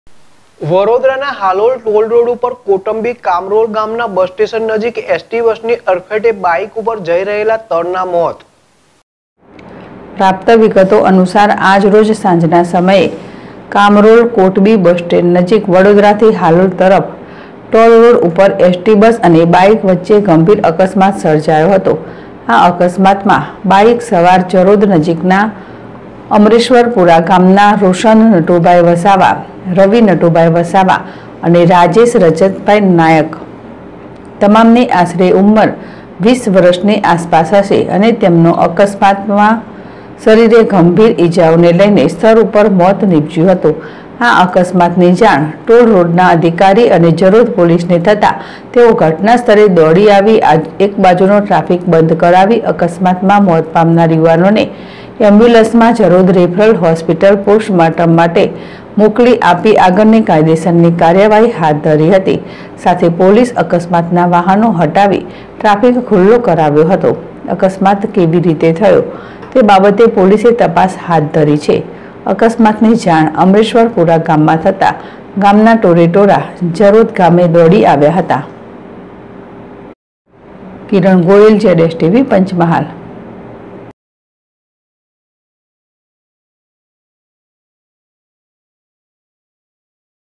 વડોદરા હાલોલ ટોલ રોડ ઉપર કોટંબી કામરોલ ગામનાં બસ સ્ટેન્ડ નજીક એસટી બસ નાં અડફેટે
વડોદરા હાલોલ ટોલ રોડ ઉપર કોટંબી કામરોલ ગામનાં બસ સ્ટેન્ડ નજીક એસટી બસ નાં અડફેટે બાઇક સવાર ત્રણનાં મોત